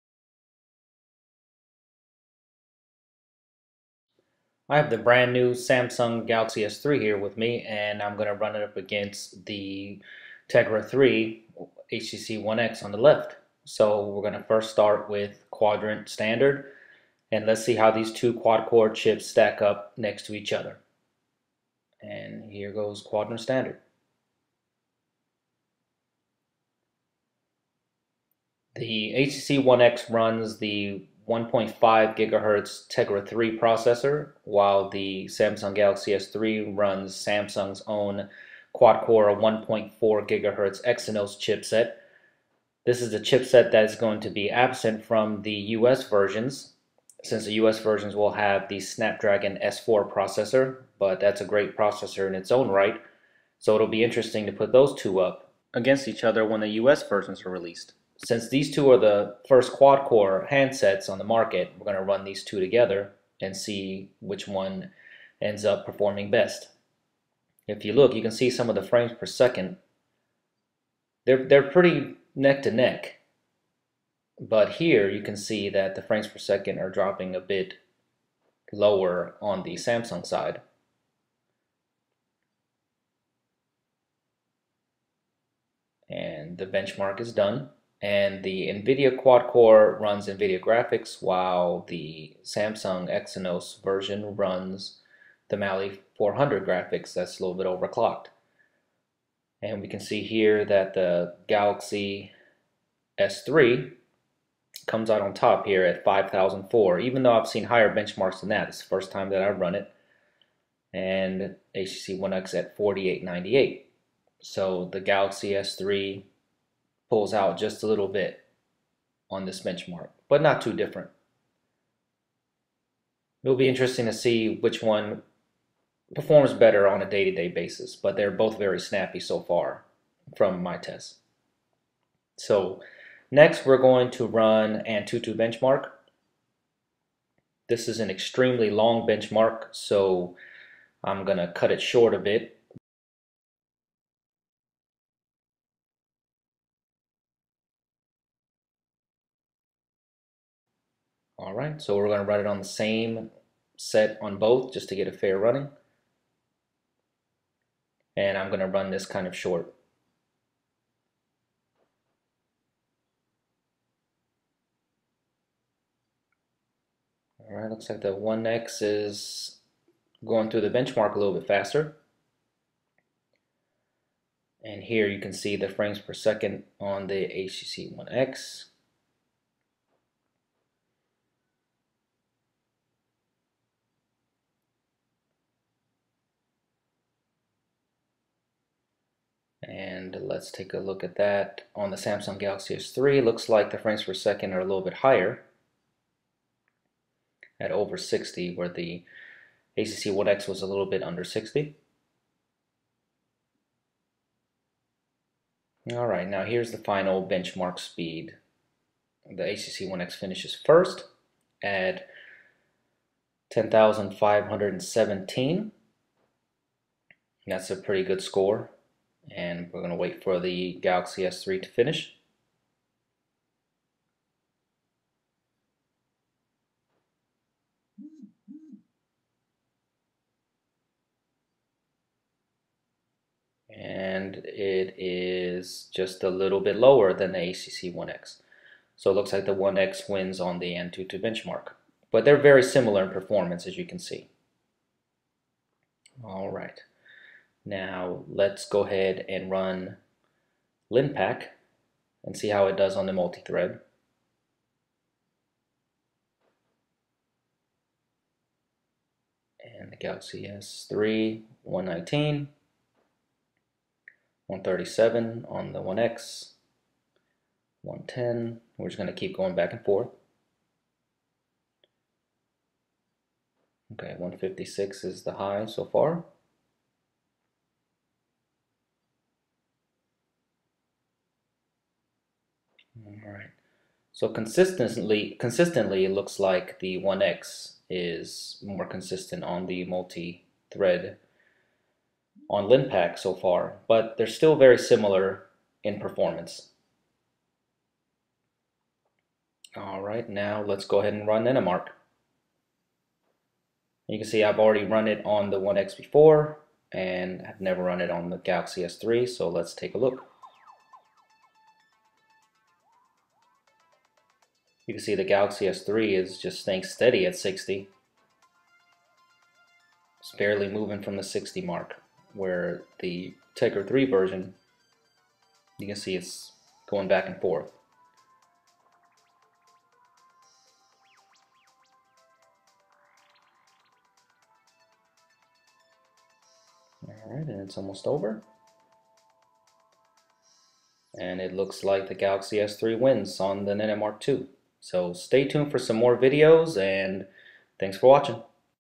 Samsung Galaxy S 3 vs HTC One X Quad Core Benchmark Test
Two of the most powerful handsets both rocking Quad Core Processors.  The HTC one X sports a Tegra 3 NVIDIA Quad Core Chipset.  The Galaxy S 3 features Samsung's own 1.4 GHZ Exynos Quad Core chipset.  Which one is faster?  Watch to find out.  Thank you for your support and there is a lot more to come!